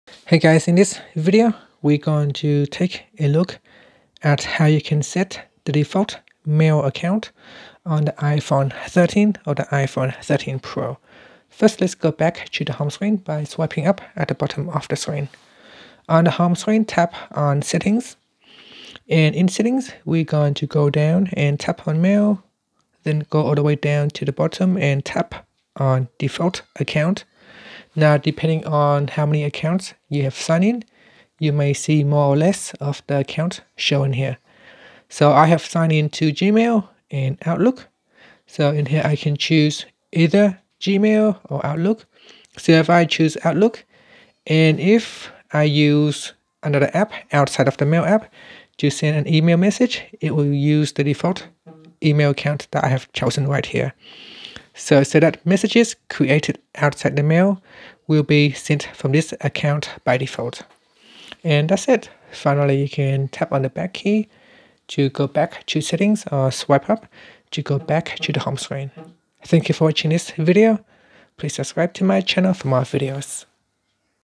iPhone 13/13 Pro: How to Set Default Mail Account
Learn how you can set the default mail account on the iPhone 13 / iPhone 13 Pro.